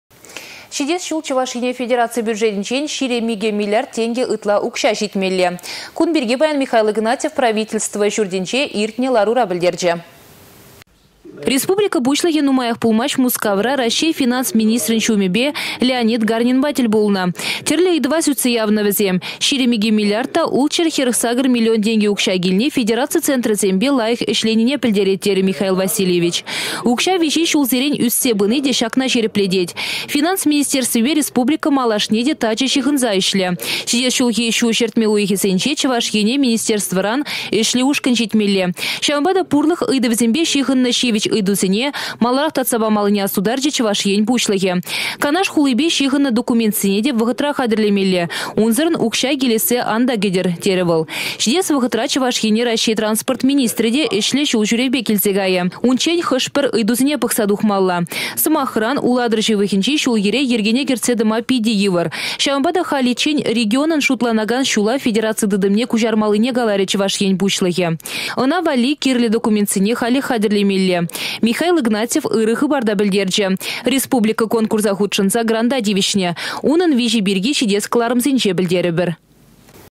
Федераци бюджетĕнчен 22 миллиард тенкĕ ытларах укçа çитмелле
Çитес çул Чăваш Ене федераци бюджетĕнчен 22 миллиард тенкĕ ытла укçа çитмелле. Кун пирки паян Михаил Игнатьев Правительство çуртĕнче иртнĕ ларура пĕлтерчĕ.